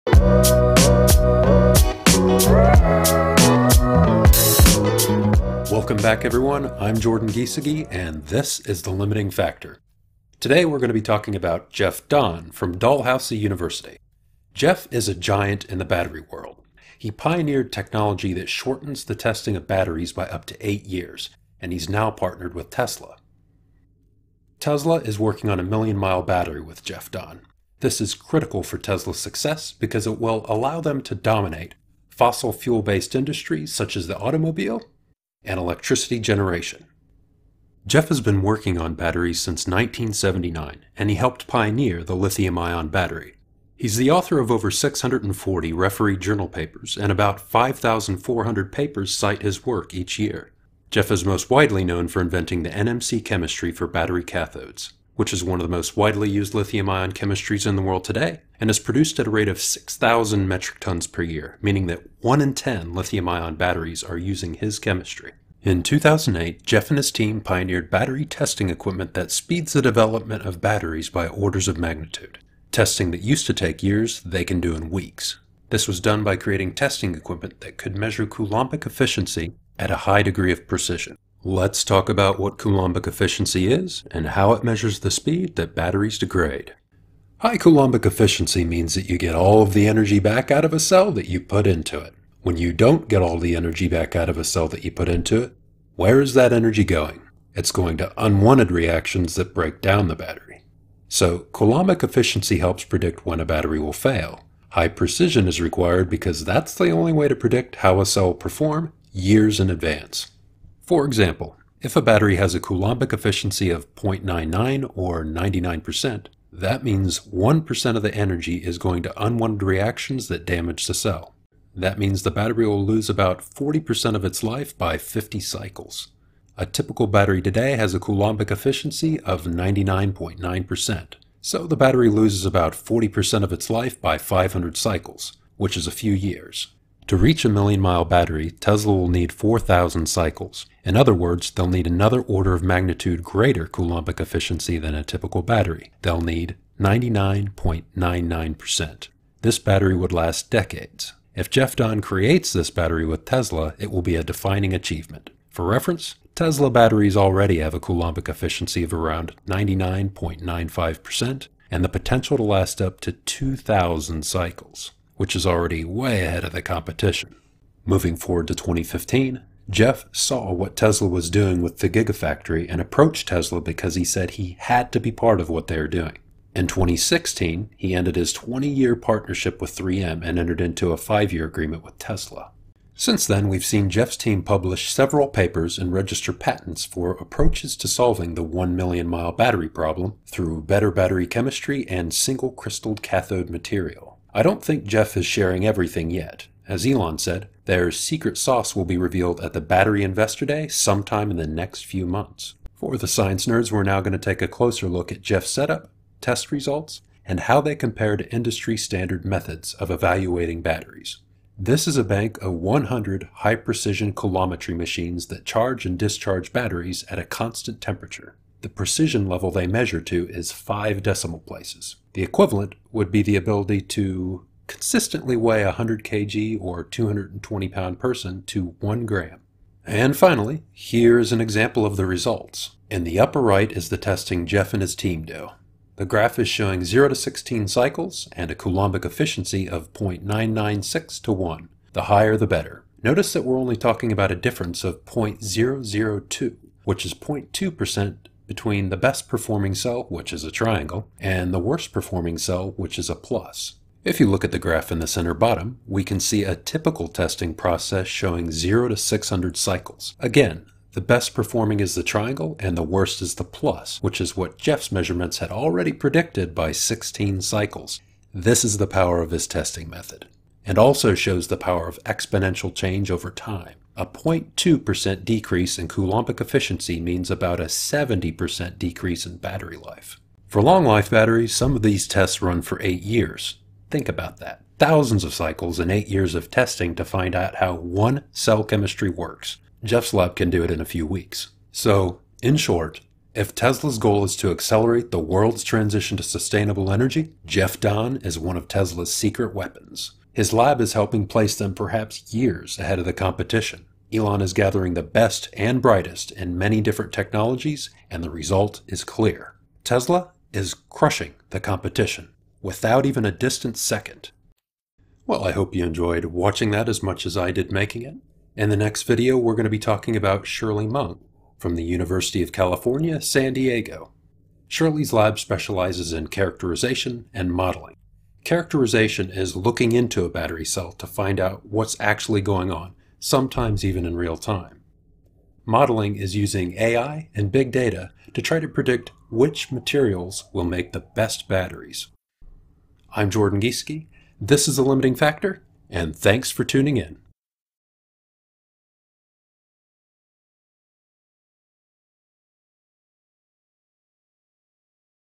Jeff Dahn - Tesla Partner and Elon Musk's Secret Weapon
In this video I talk about Jeff Dahn's relationship to Tesla and how his lab is an accelerator for battery development.

Jeff Dahn is responsible for inventing the NMC material in battery cathodes, pioneering lithium ion batteries, and developing high precision coulometery.

What is high precision coulometery?  If we were to test batteries the way they are used in the real world, it would take years.  High precision coulometry can tell us how a battery is expected to perform after just a few weeks.  This will accelerate the world's transition to sustainable energy by several years.